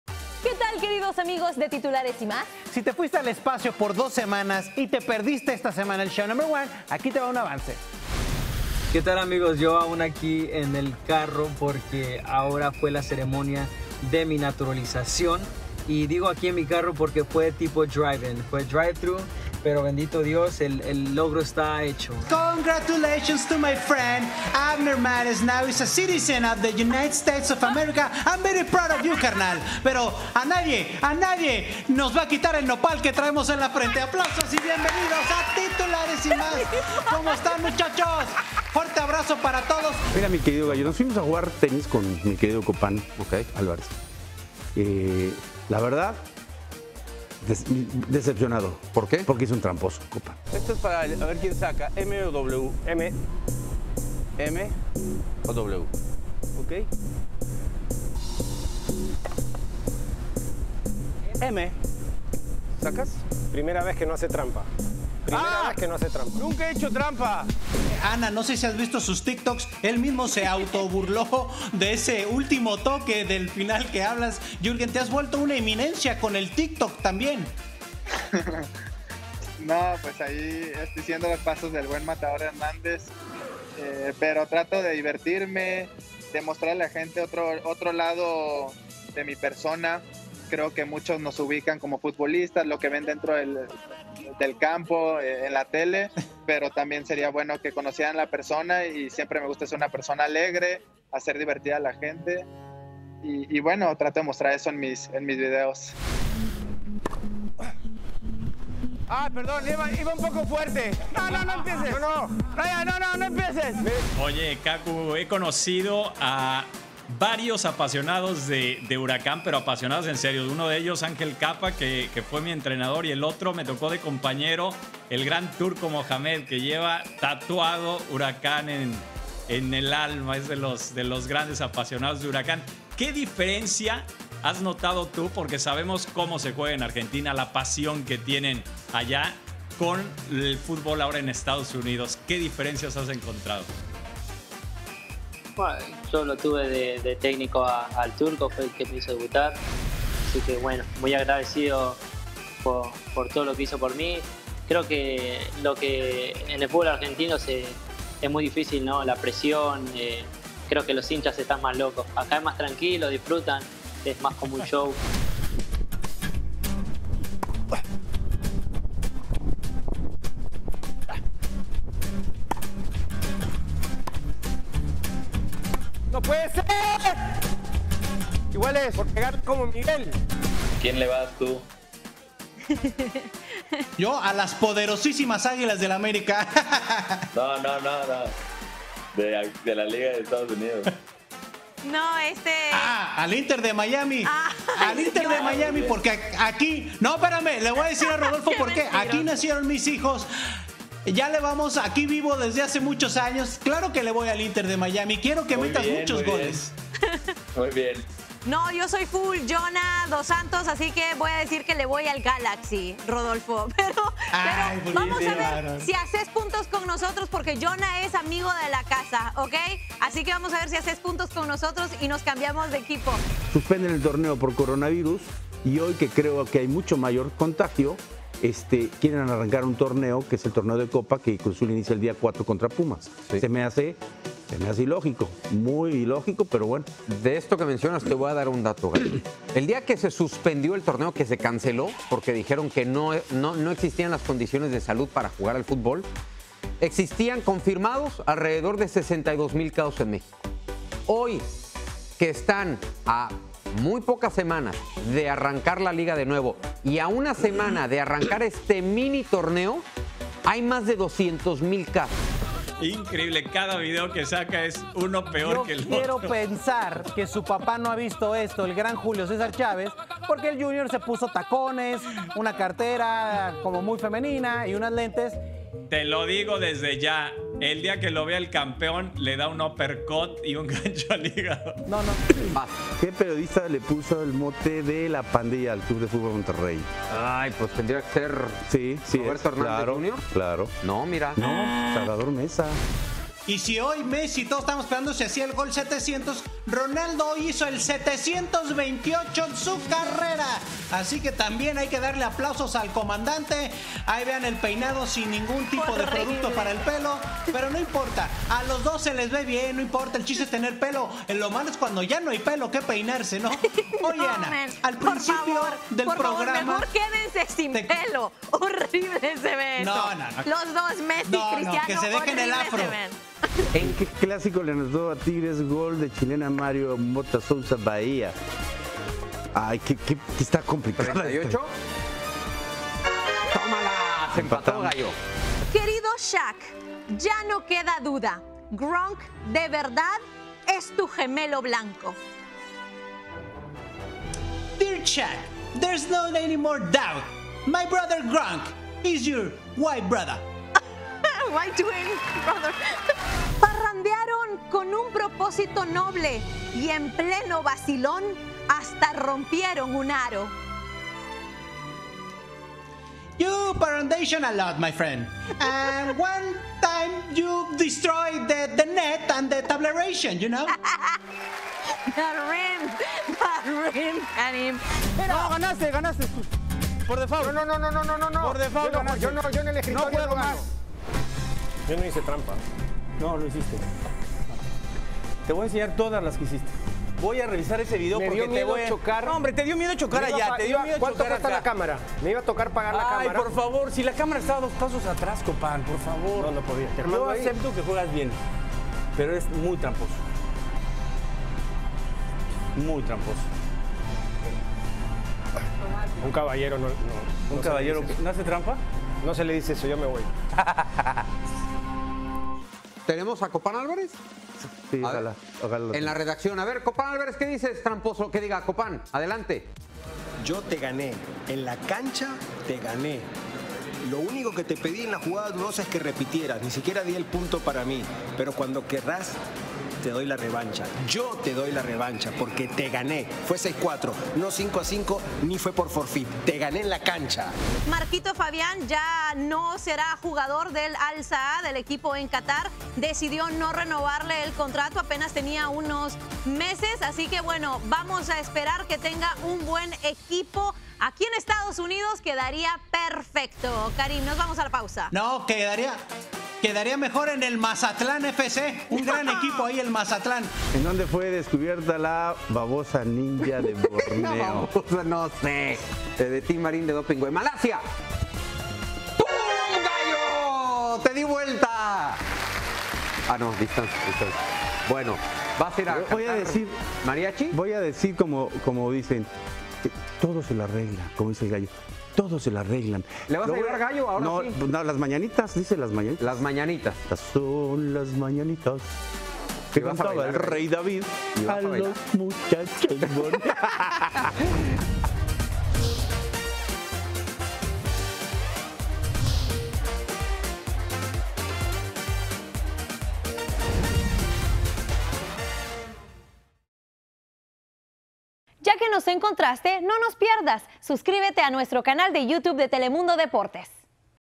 Jürgen Damm, la nueva eminencia del Tik Tok | Telemundo Deportes
Video oficial de Telemundo Deportes. El nuevo jugador del Atlanta United es la nueva revelación con la app que tiene de cabeza al mundo.